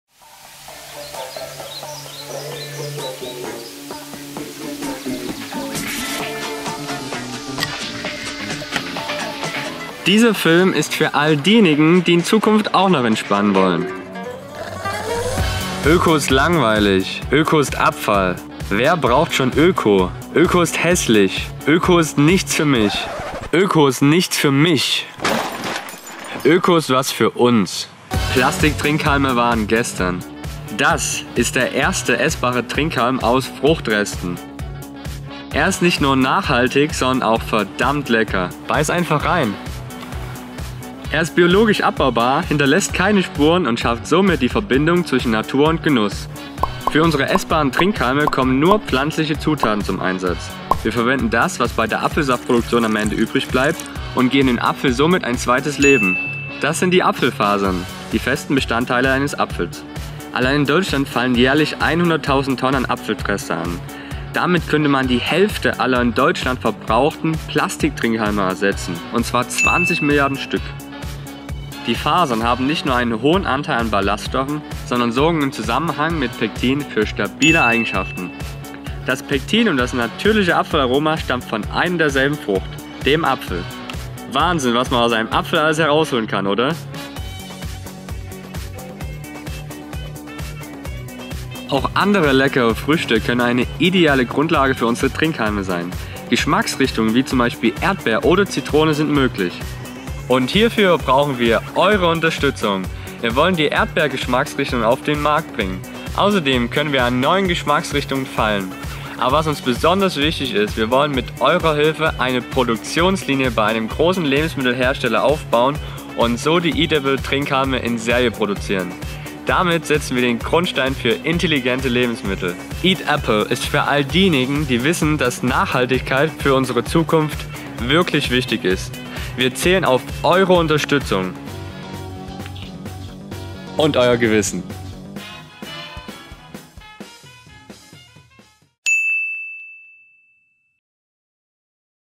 Der Erste Essbare Trinkhalm auf Fruchtbasis - Crowdfunding
Dies ist das offizielle Video zu unserer Crowdfunding Kampagne über Startnext.
Wir hoffen auf eure Unterstützung :)